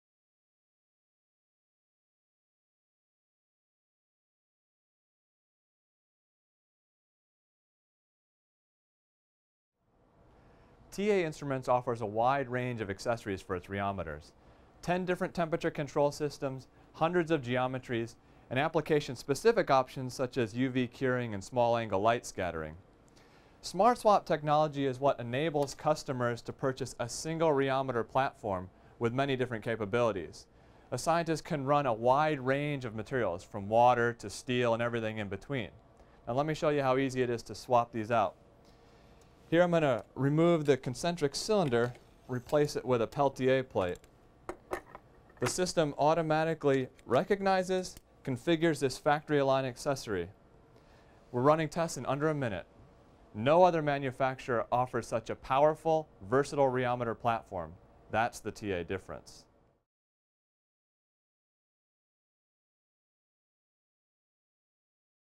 SmartSwap - Only Available on TA Instruments Rheometer Systems
Check out how quick and easy it is to change SmartSwap temperature control systems for the TA rheometer